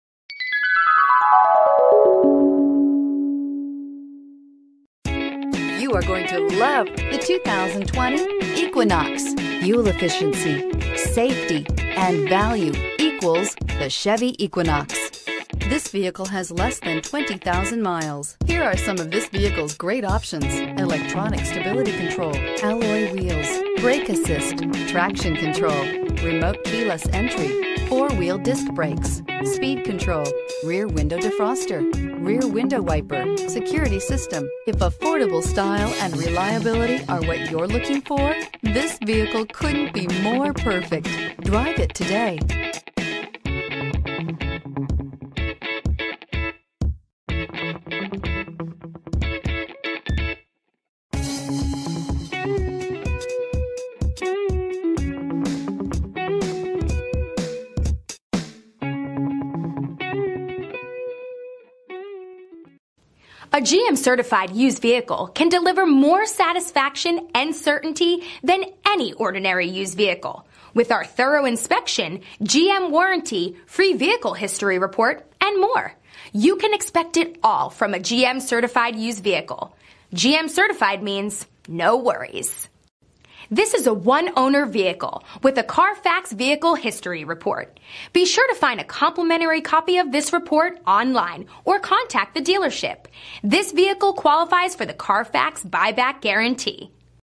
2020 Chevrolet Equinox 046655A
2020 Chevrolet Equinox LS

2020 Chevrolet Equinox LS FWD 1.5L DOHC 17 Aluminum Wheels, 2 Rear USB Charging-Only Ports, 2 USB Ports & Auxiliary Input Jack, 2-Way Power Driver Lumbar Control Seat Adjuster, 6 Speaker Audio System Feature, 8-Way Power Driver Seat Adjuster, All-Weather Floor Mats, Apple CarPlay/Android Auto, Bluetooth For Phone, Cargo Mat, Deep-Tinted Rear Glass, Fully automatic headlights, Interior Protection Package (LPO), LS Convenience Package, Preferred Equipment Group 1LS, Premium Cloth Seat Trim, Radio: Chevrolet Infotainment 3 System w/AM/FM, SiriusXM Radio Delete, Spoiler.